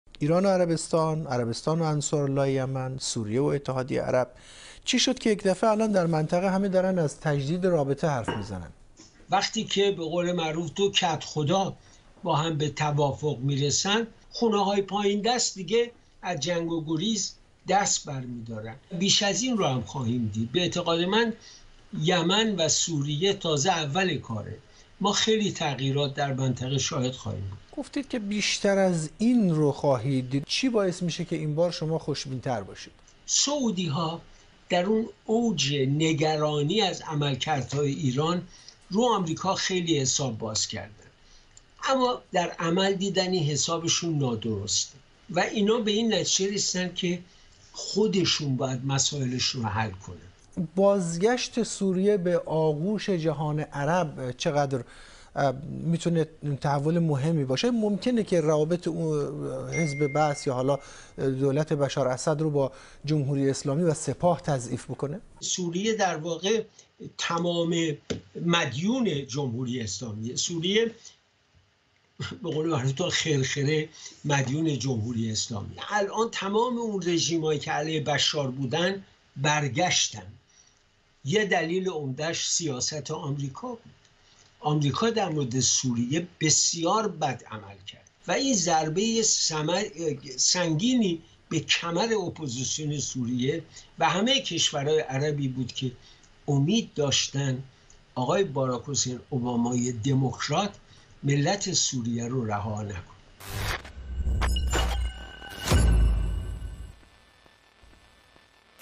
🎥 اظهارات بی‌سابقه علیرضا نوری‌زاده کارشناس ضد ایرانی از توافق تهران_ریاض
بعد از احیای روابط میان ایران و عربستان شاهد تحلیل‌های کاملا جدیدی از سوی برخی کارشناسان رسانه‌های فارسی زبان از جمله علیرضا نوری زاده هستیم که تا چندی پیش این کارشناس مواضع بسیار تندی در دفاع از عربستان سعودی علیه ایران